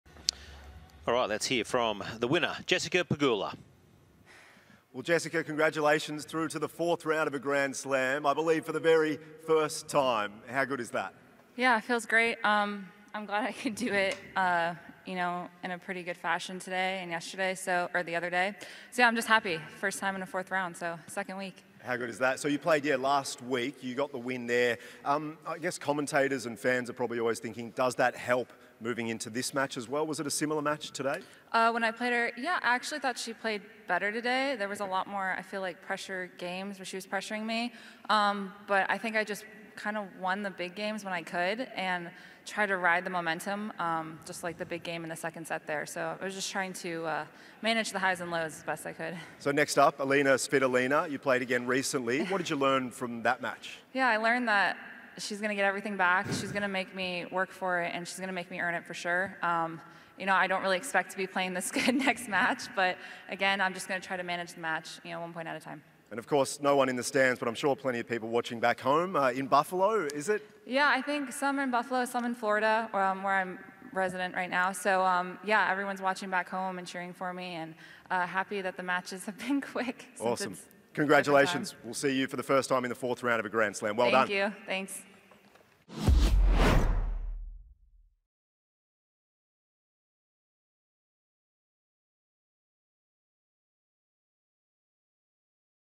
Jessica Pegula: "She's gonna make me earn it!" (3R) on-court interview | Australian Open 2021
Jessica Pegula's on-court interview following her third round victory over Kristina Mladenovic at the 2021 Australian Open.